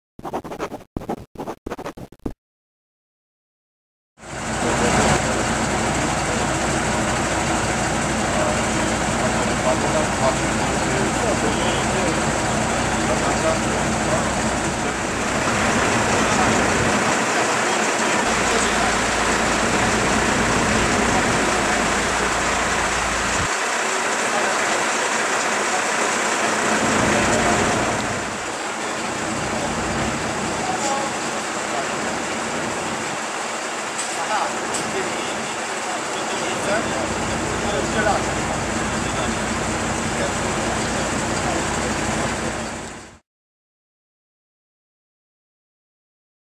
Pożar piwnicy przy ul. Stolarskiej w Gdyni - 8.10.2012r.
Krótko po godzinie 22:00 dyżurny Straży Pożarnej odebrał telefon o zaprószeniu ognia przy ulicy Stolarskiej 18. Na miejsce zostały wezwane dwie jednostki gaśnicze.

Jak udało nam się ustalić, do zdarzenia doszło w piwnicy bloku podczas...